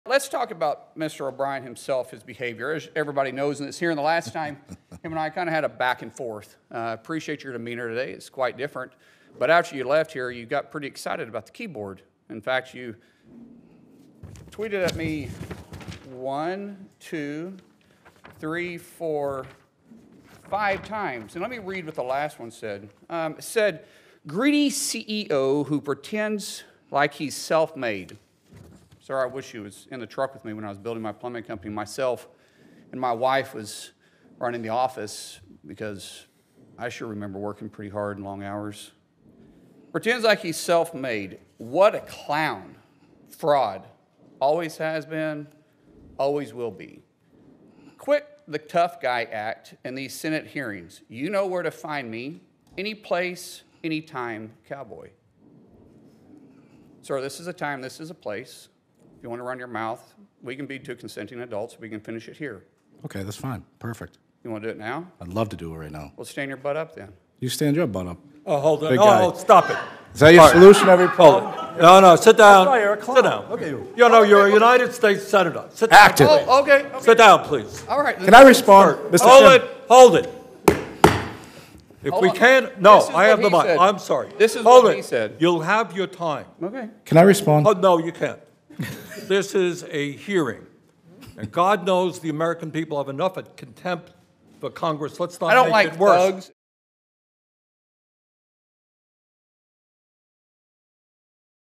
Sen. Mullin Challenges Teamsters President to a Fight During Hearing | WSJ News
Sen. Markwayne Mullin called out Teamsters President Sean O’Brien during a Senate hearing on Tuesday for posts O’Brien published about Mullin on X (formerly Twitter) and suggested he and O’Brien settle the matter with a fight. Photo: Elizabeth Frantz/Reuters